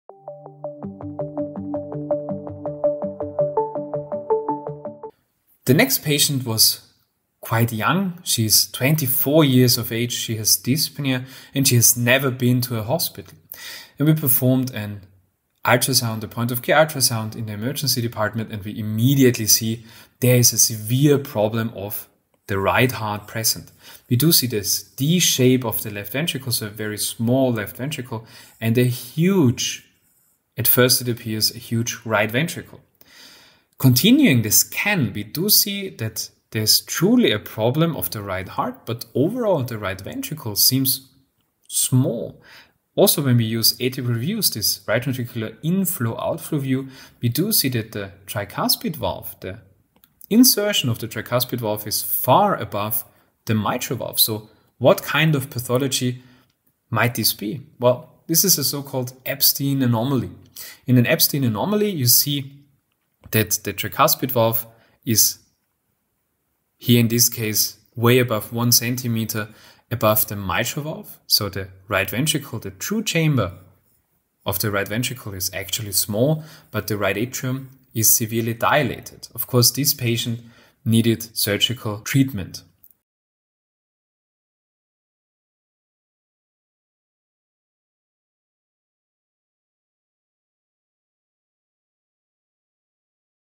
〖Echocardiography〗 Ebstein anomaly 💘
A congenital abnormality of the RV is the Ebstein anomaly (the TV inserts too far into the RV, creating a huge RA). In this young female, Emma, it was described when she visited an ER with dyspnea❣️